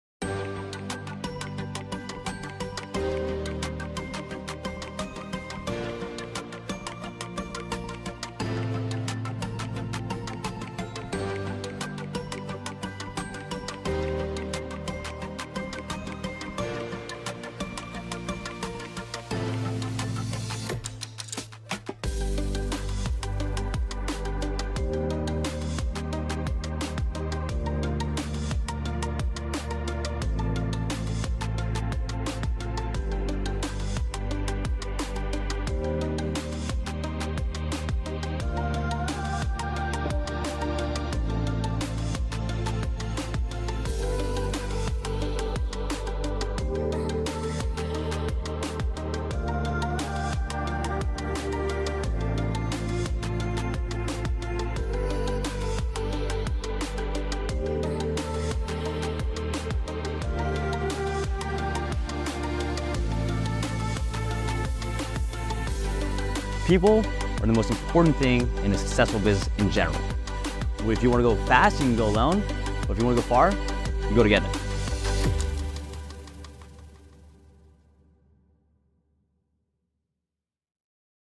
Zen Coast University - Commercial
Are you ready to embark on an inspiring Real Estate Investing Journey?
Look no further! At Zen Coast University, we have life changing programs that'll transform the way you invest in real estate. 🏡💼💰

🌅 Discover the secrets of successful real estate investing while enjoying the calming waves of knowledge and support from our incredible community. 🌊📚 With expert guidance, powerful resources, and a tribe of like-minded investors, you'll be equipped to navigate the market like a pro!

🙌 Join us as we redefine the art of real estate investing – the Zen way! Find your balance between growth and serenity, and let's ride the tides of success together. 🚀🌊 Don't miss this chance to create your own success story! 💪💭